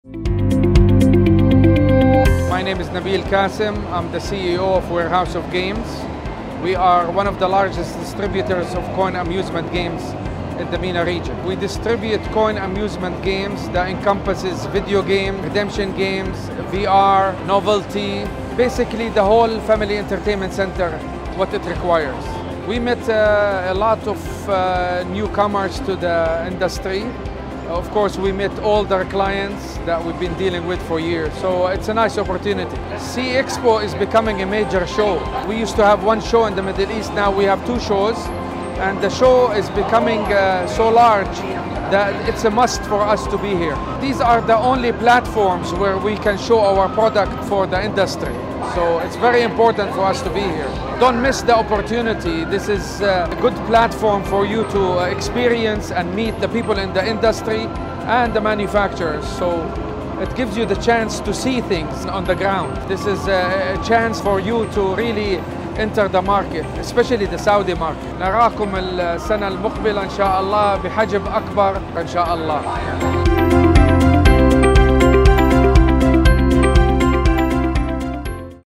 Warehouse of Games meets key buyers and newcomers to the industry at SEA Expo!
Venturing into its 6th edition, Saudi Entertainment and Amusement (SEA) Expo 2023 experienced exponential growth having doubled footfall in comparison to last year.

The event surpassed all expectations, attracting a record-breaking number of attendees, esteemed speakers, high-quality exhibitors, and visionary sponsors united under one roof. Together, they set the stage for unparalleled discussions and remarkable insights within the dynamic realm of attractions and leisure.